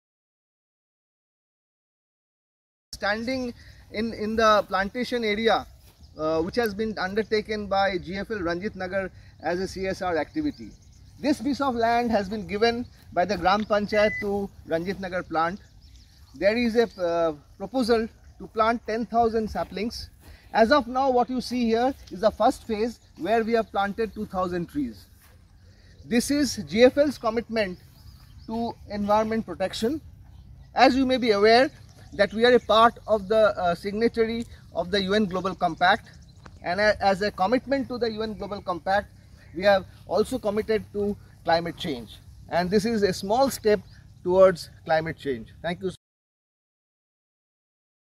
Environmental responsibility - Gujarat Fluorochemicals Ltd.
Allowing the earth’s forest to recover could soak up a significant amount of global greenhouse gas emission according to the latest findings. If the goal is to fight climate change, countries have to reverse the course on how they use the forest. As a responsible corporate citizen of the country, GFL has committed itself to be a part of the global climate change initiative. We are now a part of the Science Based Target Initiative (SBTi) and a signatory to the UN Global Compact and have undertaken various initiatives to promote greater environmental responsibility. One of them being a massive afforestation drive at Ranjitnagar in Gujarat – a small step taken towards climate change. Know More